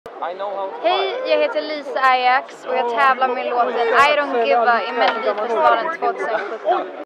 Lisa Ajax på andra chansen
Lisa Ajax på presskonferensen efter fredangens repetition inför andra chansen.